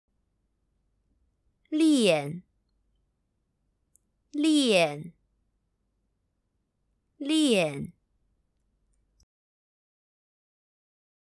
How to pronounce 链（lian）in Mandarin Chinese?
This video tells you how to pronounce    链（lian）in Modern Standard Mandarin Chinese.